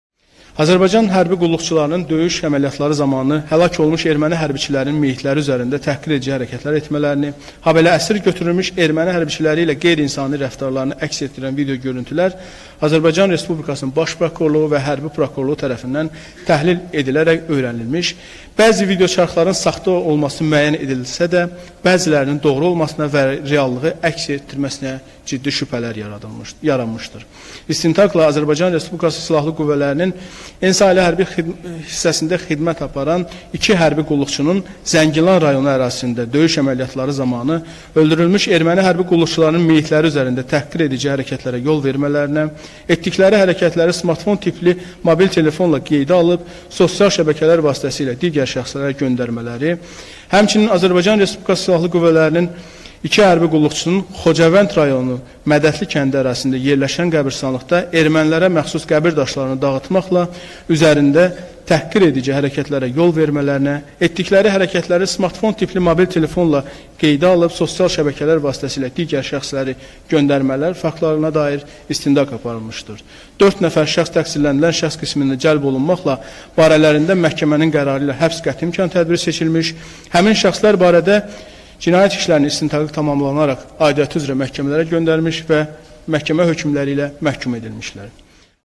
İşgəncə videolarına görə hərbçilərimiz həbs olundu - BAKU TV
İşgəncə videolarına görə hərbçilərimiz həbs olundu